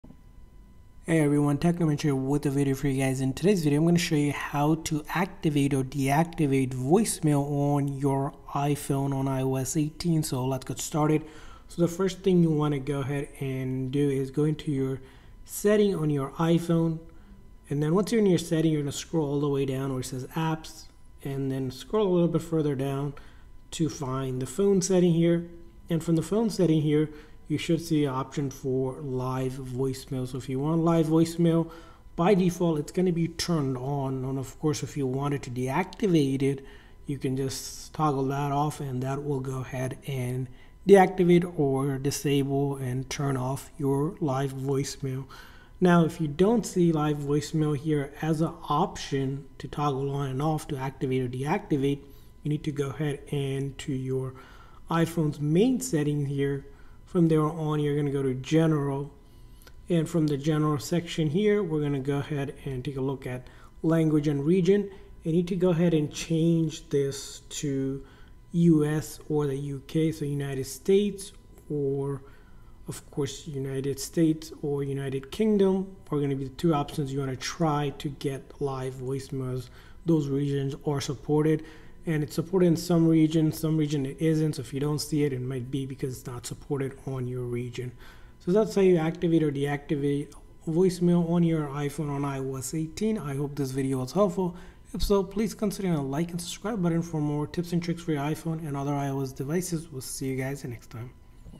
How to Activate or Deactivate Voicemail on iOS 18
In this tutorial I show you how to activate and deactivate live voicemail on iPhone on iOS 18!